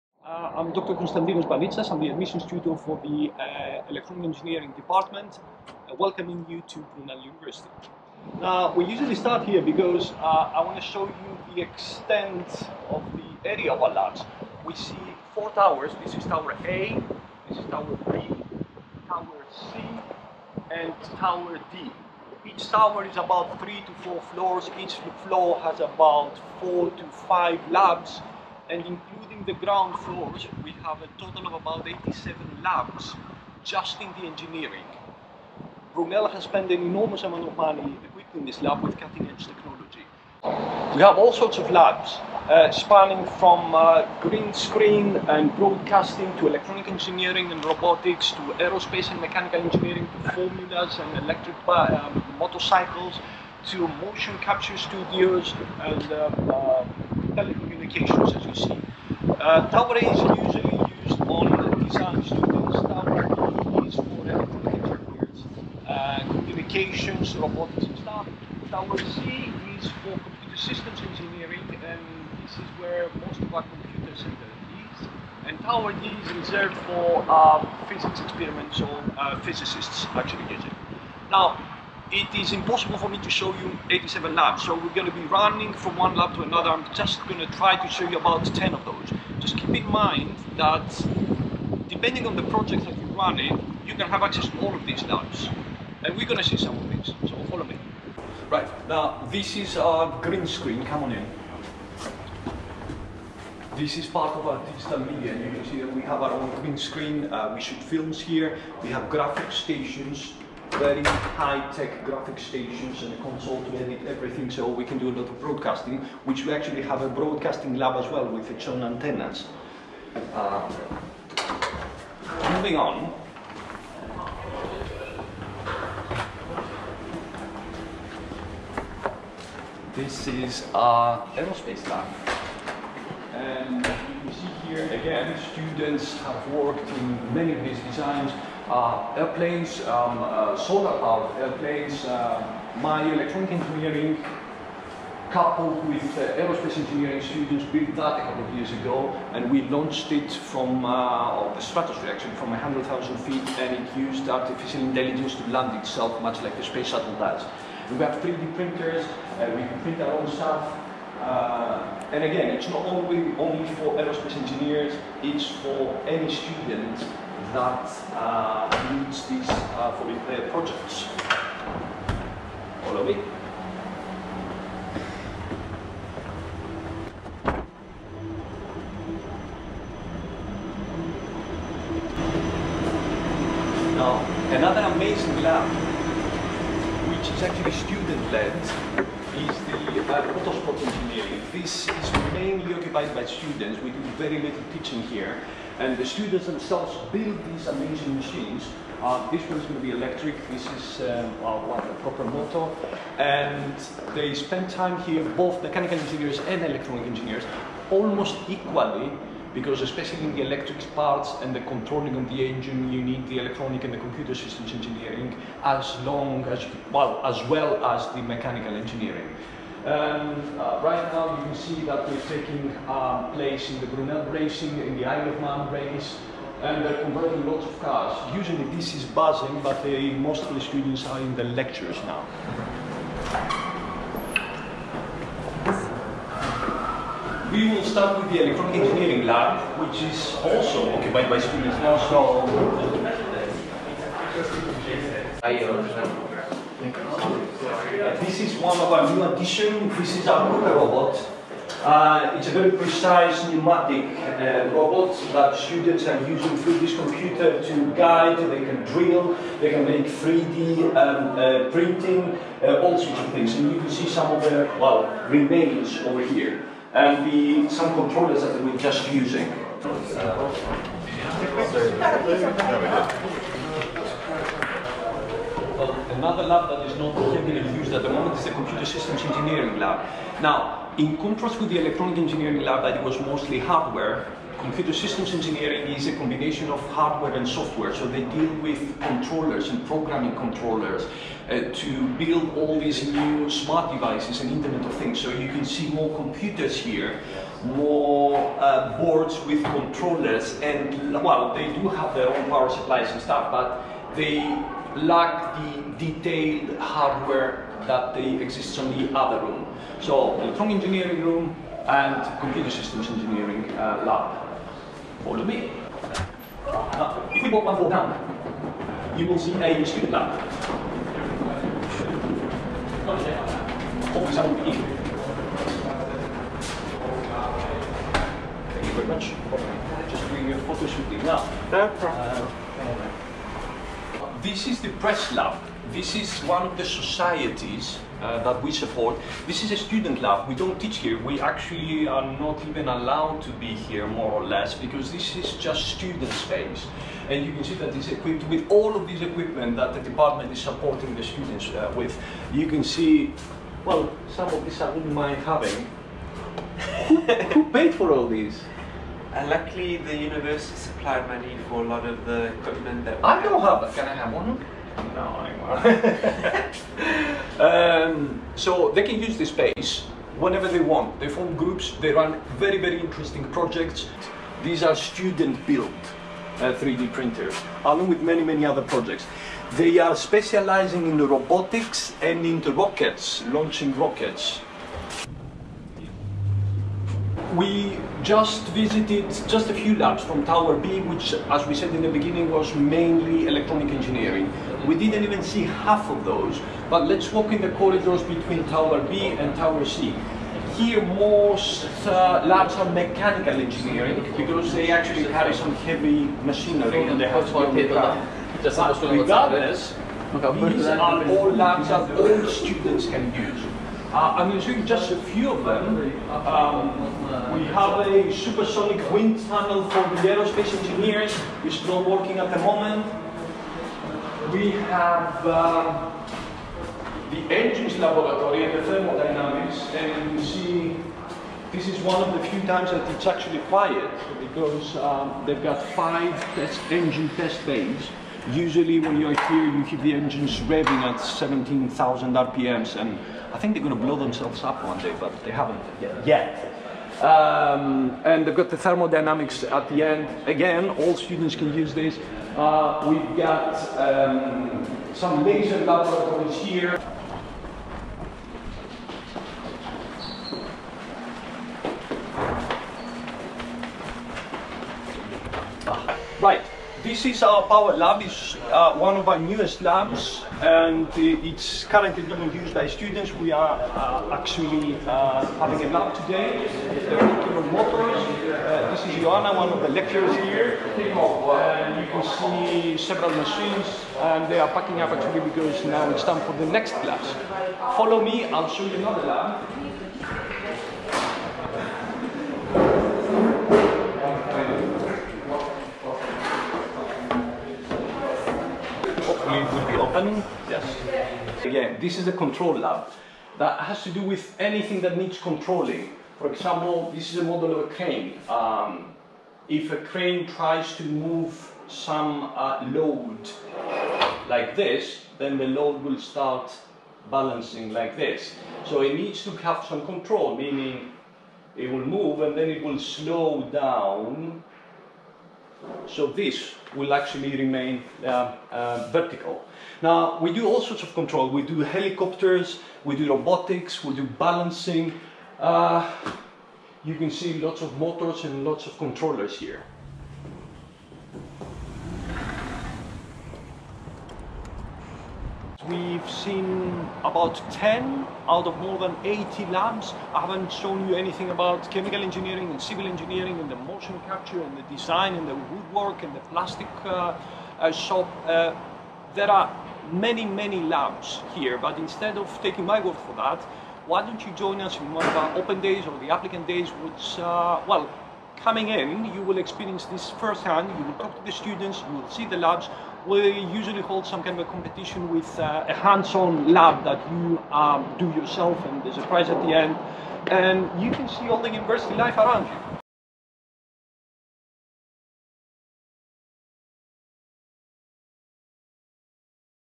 Virtual tour of Brunel University's Electronic and Electrical Engineering labs
Electronic and Electrical Engineering Admissions tutor and Senior lecturer Konstantinos Banitsas takes us on a virtual tour of the many engineering labs on our London campus.